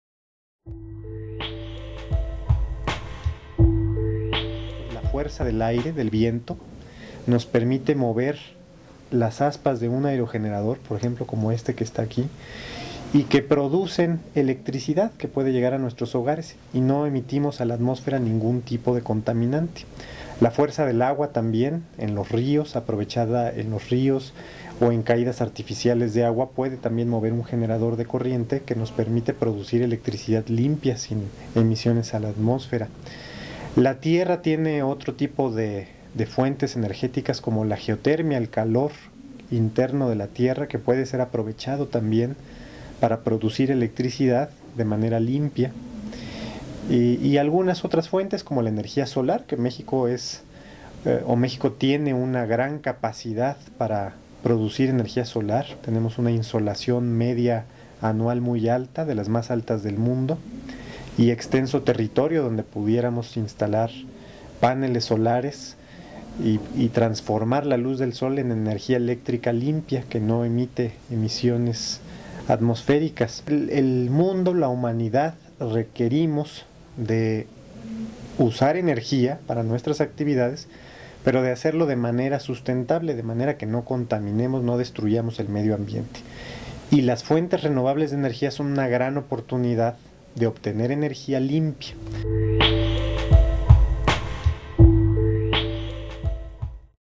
¿Qué son las energías renovables? el Diputado Diego Cobo Terrazas lo explica
El Diputado del Partido Verde Diego Cobo Terrazas habla sobre el beneficio del uso de energías renovables o limpias.